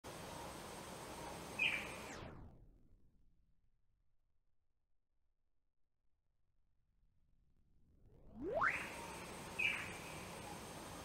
Native or domestic mouse?
We don't have too many troubles with rodents, there's enough birds of prey around to keep them to a minimum.
But every now and then one will turn up. I refuse to poison, concerns for the predatory animal that might eat a dead mouse and it's against my religion of caring for life.
I was curious if this was a domestic or native mouse so sent the video to an expert.
The verdict: what do you think??